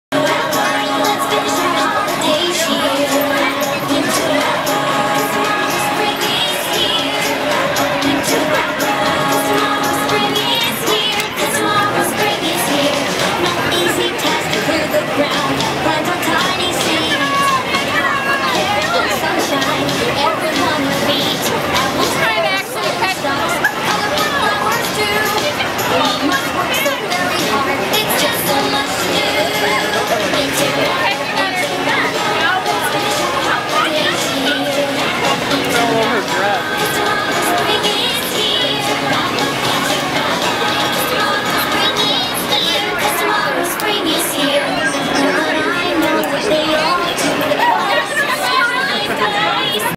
Emy's 3rd Birthday Party
Emy and Derek in the ticket booth at Chuck E. Cheese!